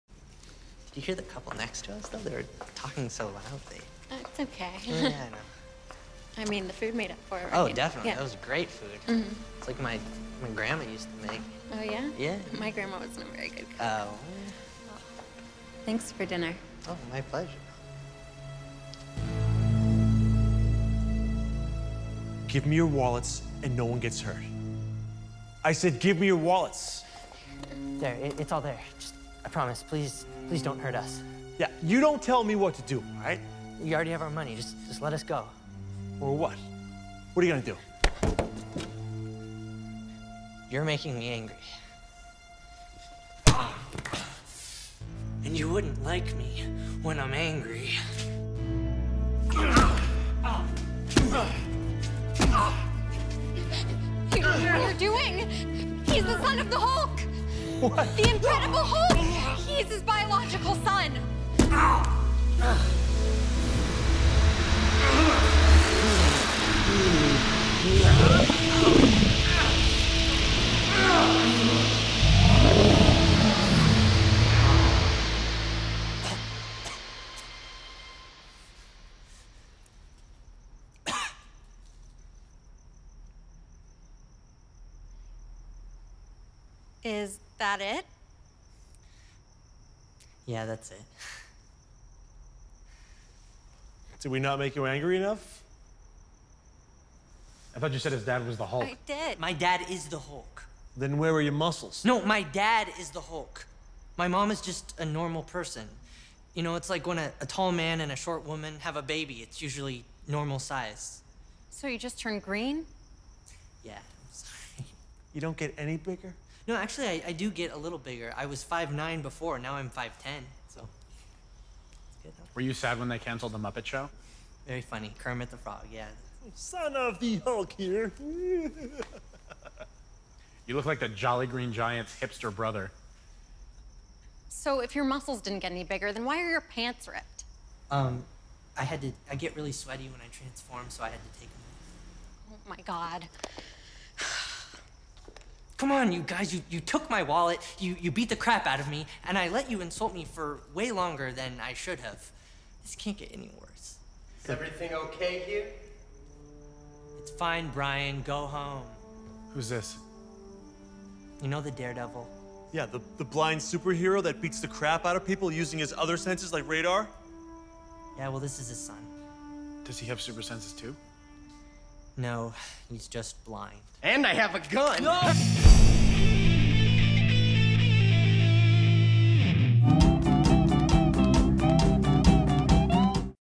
Son of the Hulk
Bruce Banner's biological son reveals what happens when you make him angry.

A Rat Pageant sketch

Starring

Mark Saul
Sean Kearney
Ilana Berger
John Kyle Grady
Jeremy Radin

Written by
Rat Pageant

Directed by
Ryan Orenstein

Edited by
Sean Kearney and Katie Orenstein

Creative Advisor and Motion Graphics
Katie Orenstein

Cameras Operators
Matt Taylor and Derek Baranowski

Production Sound
George Wymenga

Post Production Sound and Mixing
Danny Langa

Score
Michael Costantini